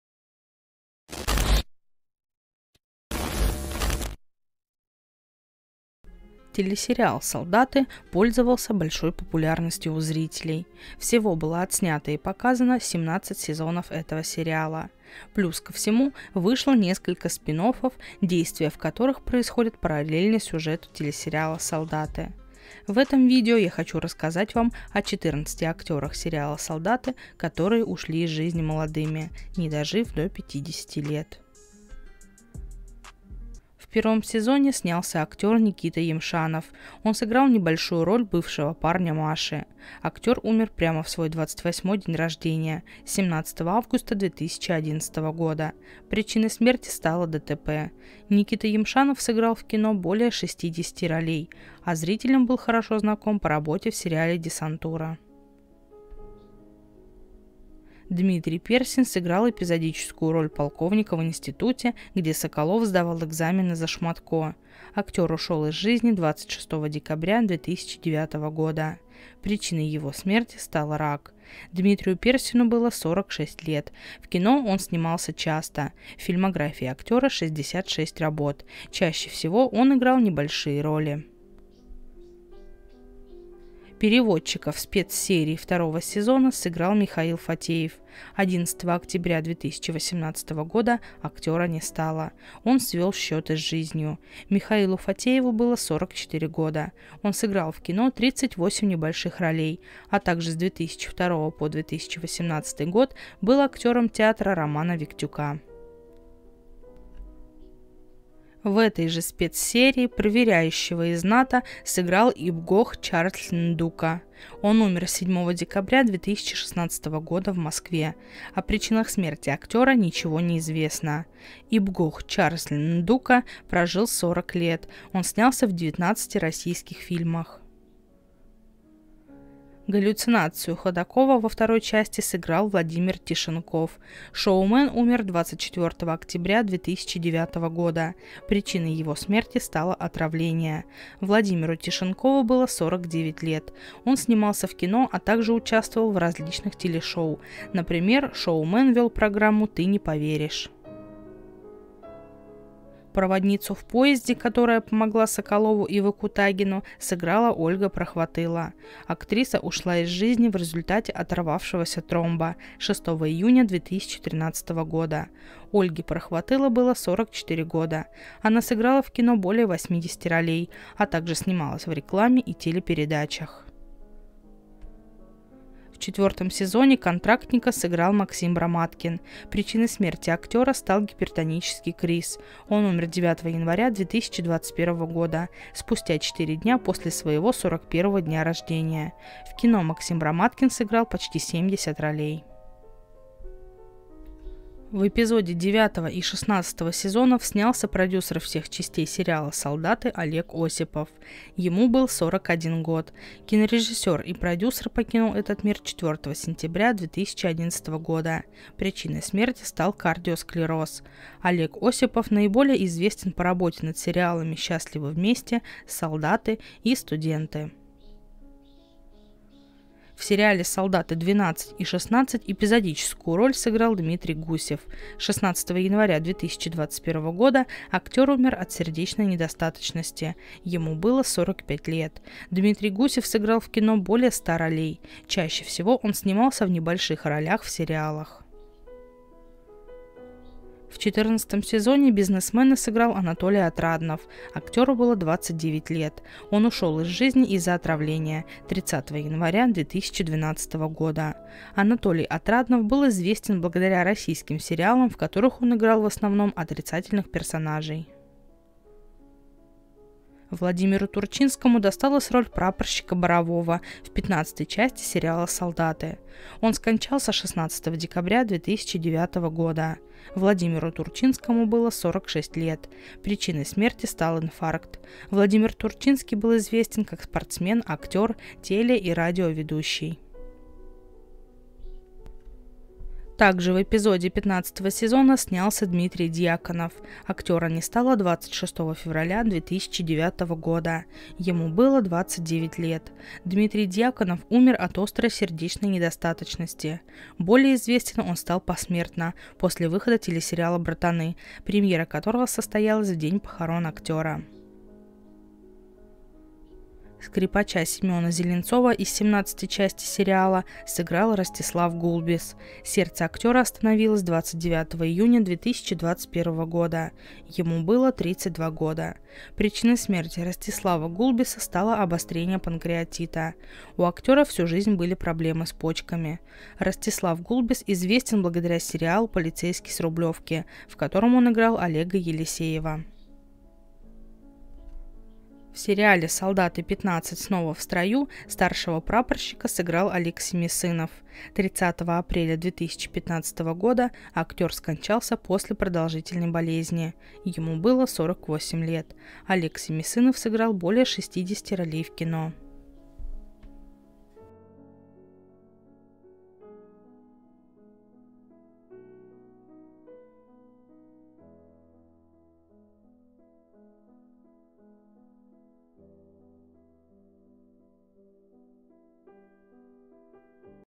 14 МОЛОДЫХ АКТЕРОВ СЕРИАЛА "СОЛДАТЫ", КОТОРЫЕ УШЛИ ИЗ ЖИЗНИ
Все комментарии модерируются!